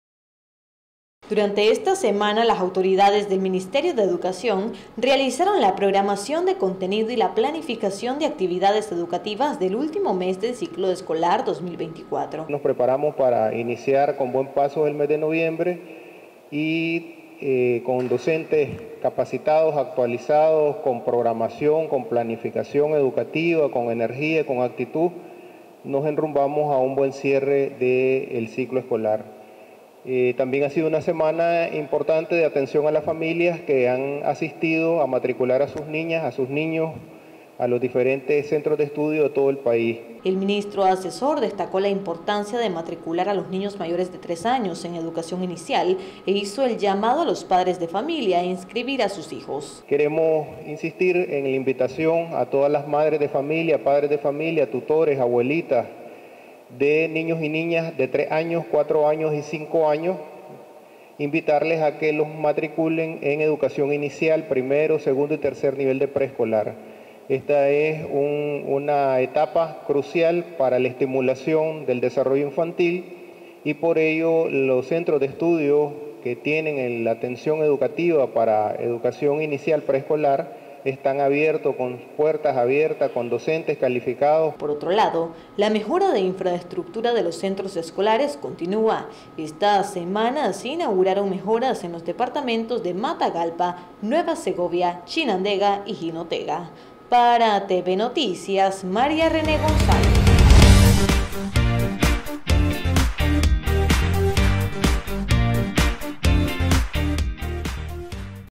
Avanza de manera exitosa el proceso de Matricula Escolar 2025
Durante esta semana las autoridades del Ministerio de Educación realizaron la programación de contenido y la planificación de actividades educativas del último mes del ciclo escolar 2024.